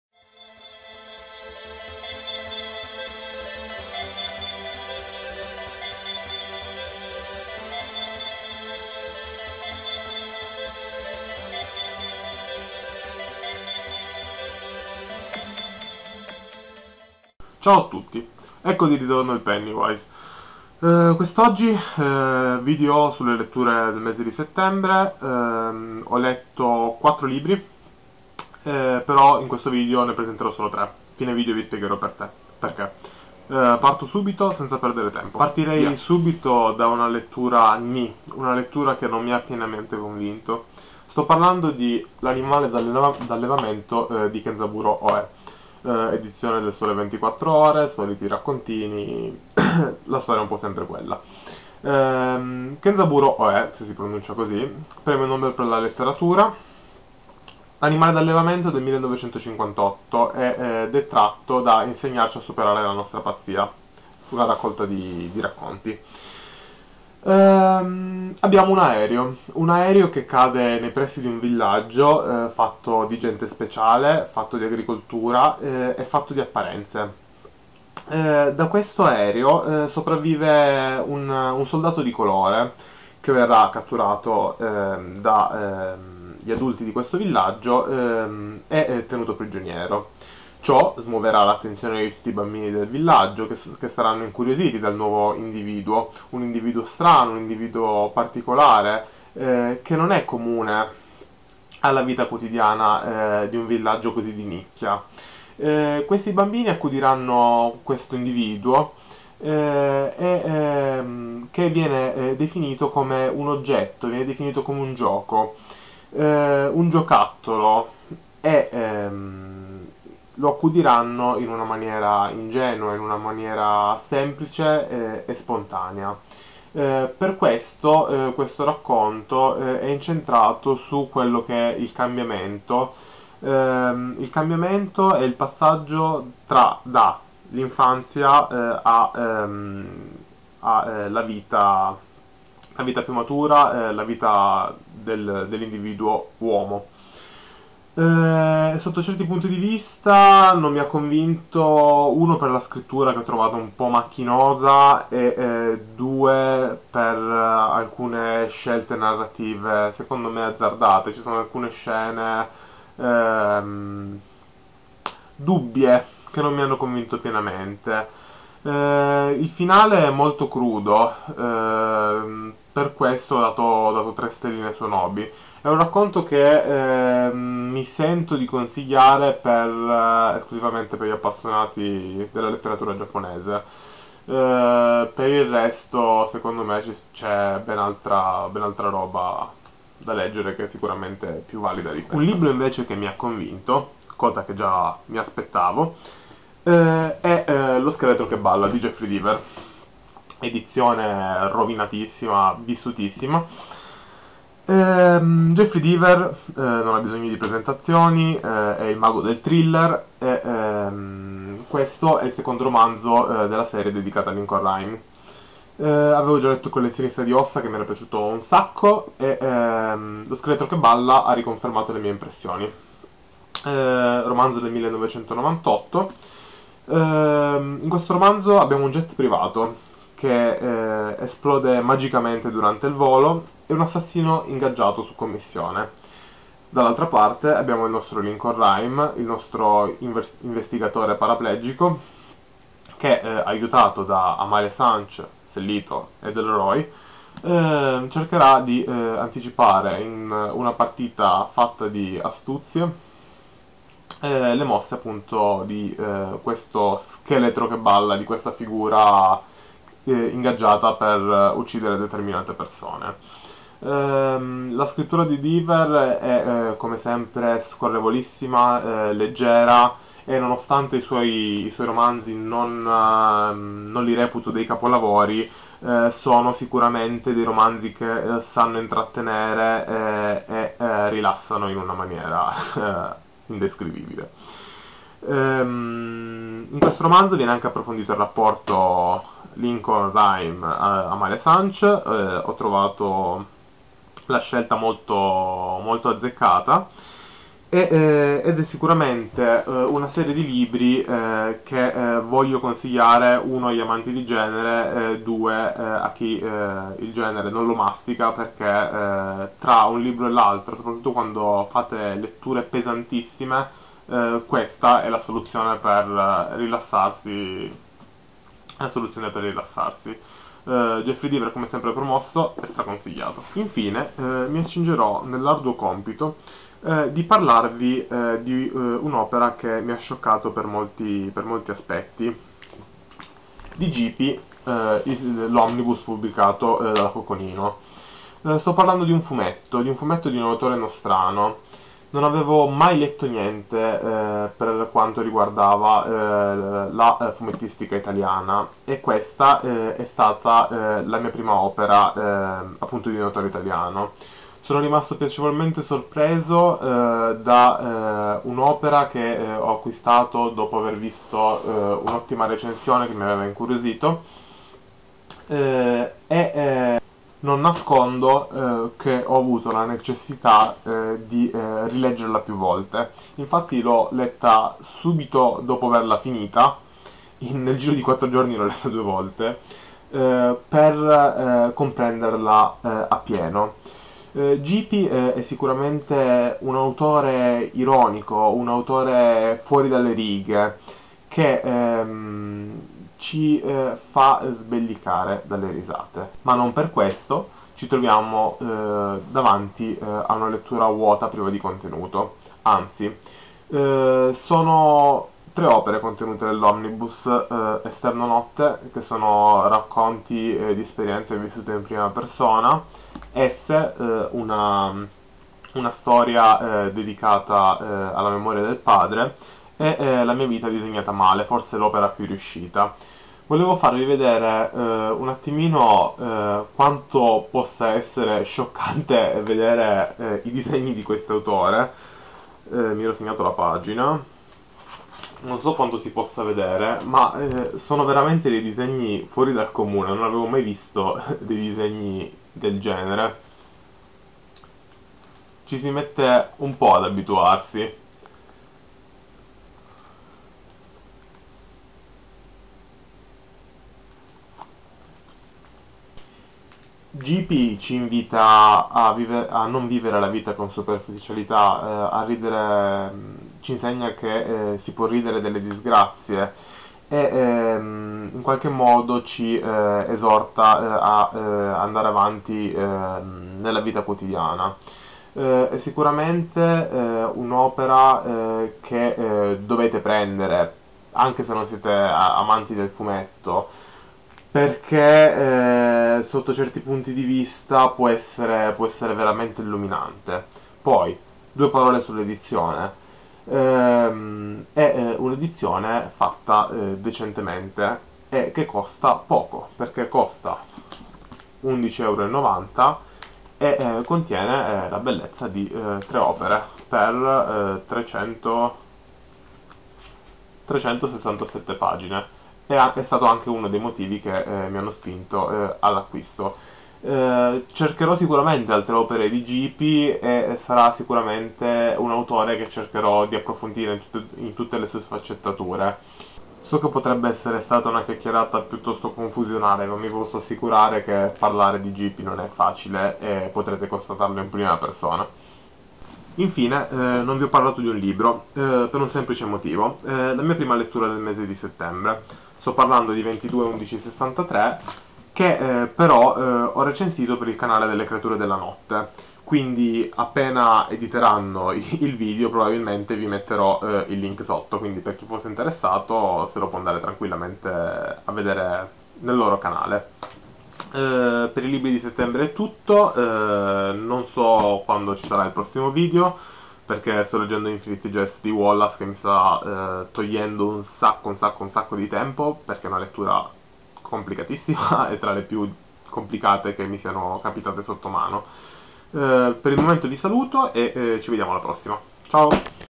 Libri Settembre 2012
LIBRI CITATI:
L'animale d'allevamento - Kenzaburo Oe
Lo scheletro che balla - Jeffery Deaver
Omnibus - Gipi (Graphic Novel)